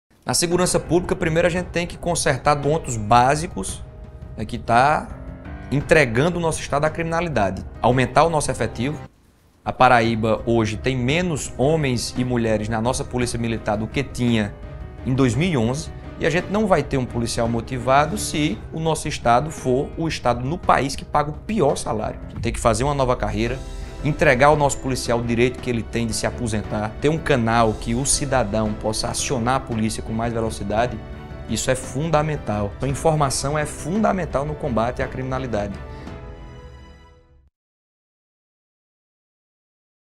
Propostas de Pedro para a Segurança Pública
Com valorização do policial, uso da tecnologia a nosso favor e pulso firme, vamos fazer a Paraíba recuperar a marca de ser um estado tranquilo.